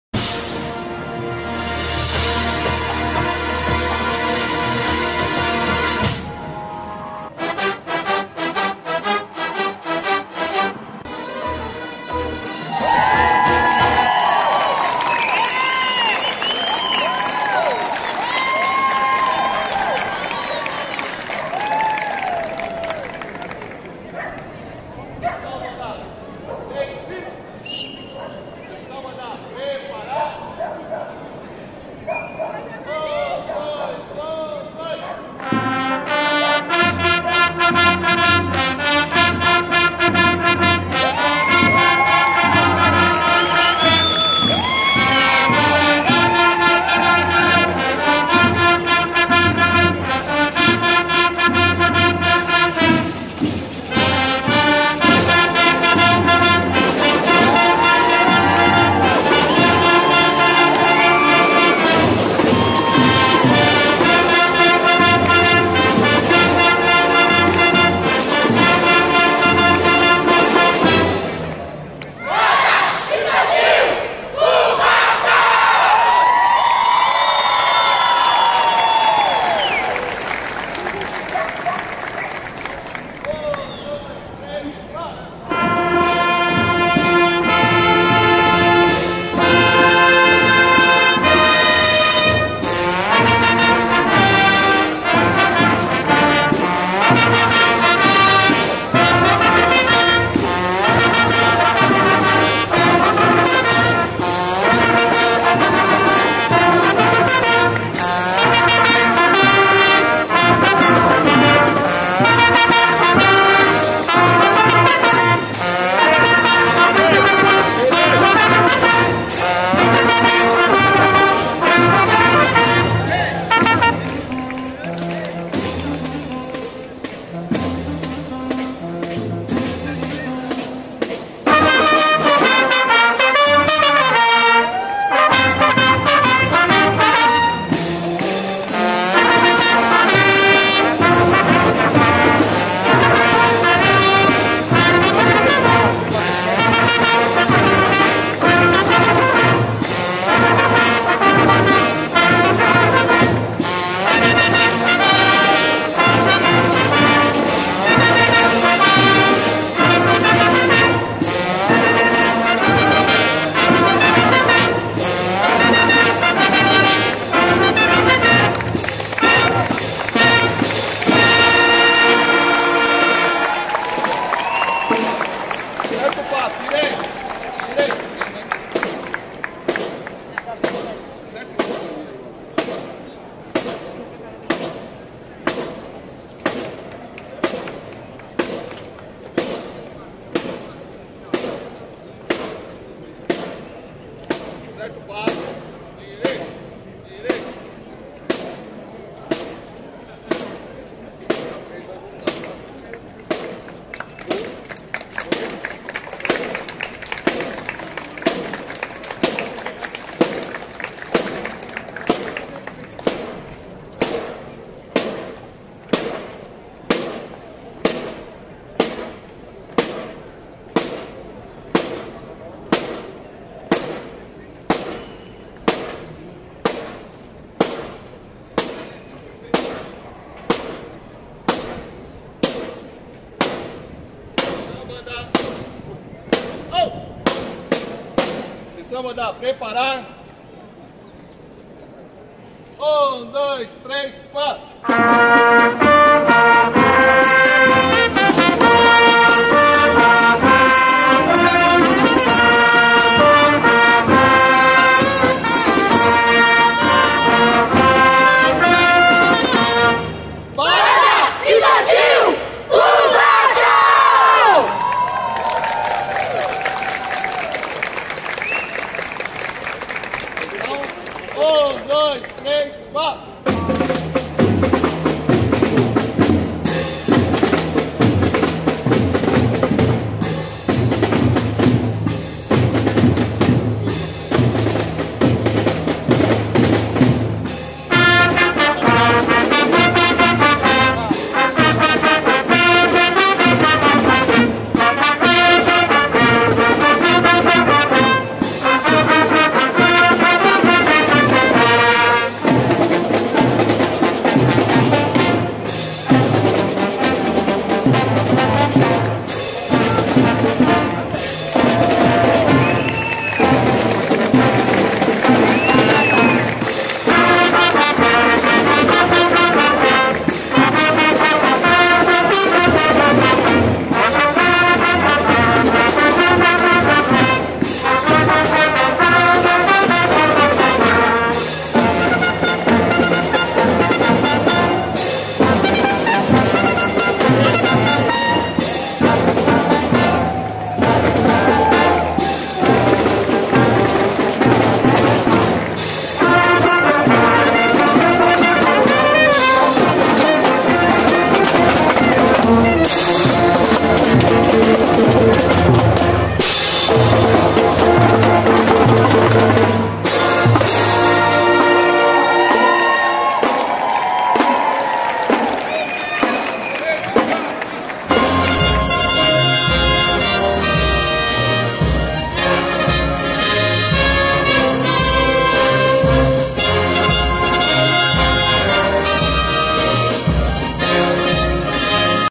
Banda Marcial Infantil de Cubatão 2019 no 4° Concurso de Bandas e Fanfarras de Santos 2019
Banda Marcial Infantil de Cubatão 2019

A banda infantil de Cubatão fez sua apresentação especial na cidade de santos abrilhantando ainda mais o concurso.

4° Concurso de Bandas e Fanfarras de Santos 2019
Realizado no dia 07/07/2019

Nossa Parceria:
UNIBANDAS TV